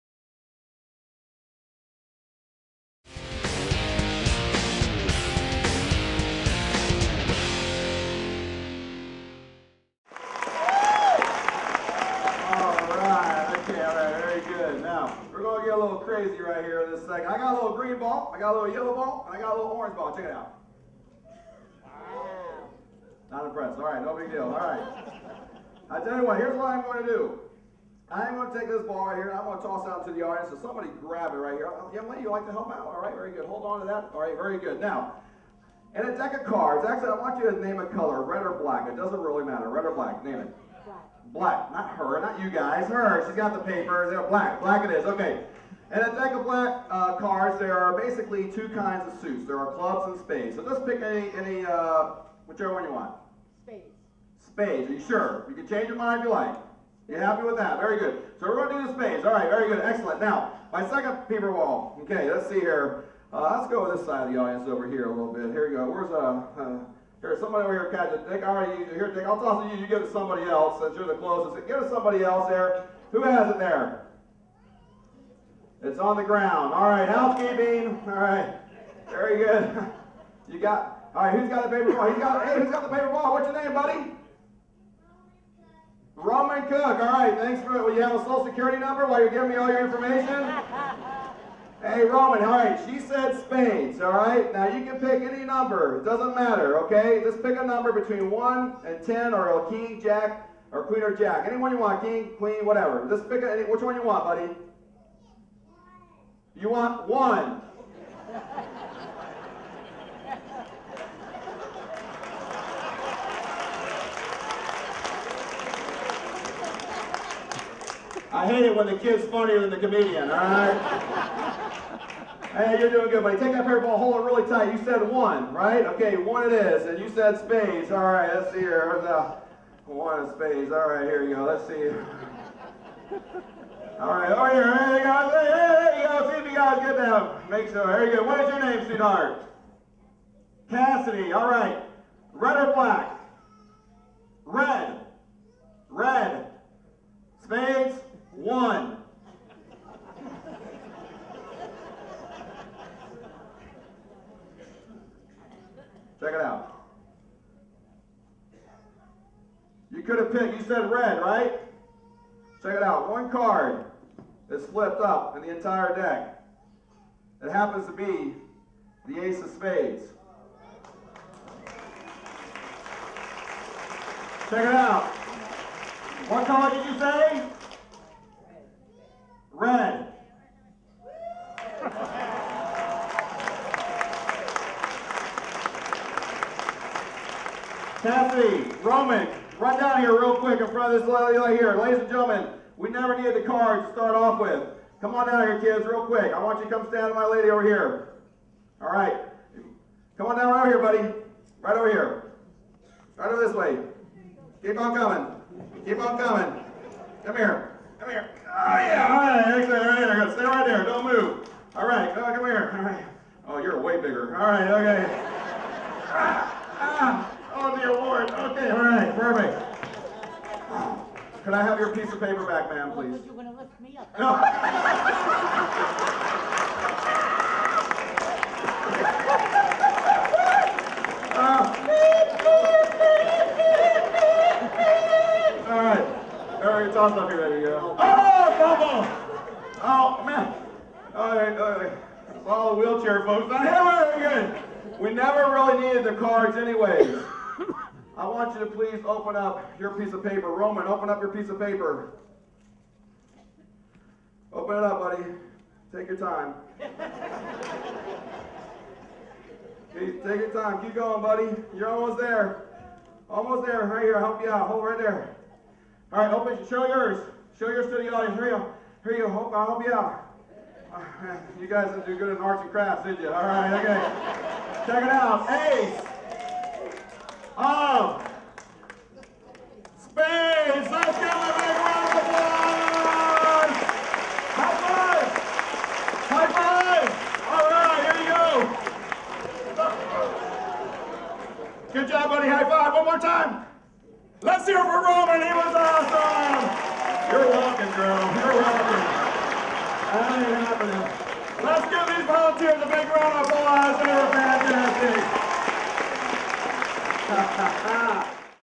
Glen Foster-Killer Prediction 9-2011
Part of Glen Foster's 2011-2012 Show titled, "I AM MENTAL."
I AM MENTAL is a Comedy Mind Reading Show that runs 75 minutes. It is full of audience participation and comedic situations that arise from Comedy Mentalism.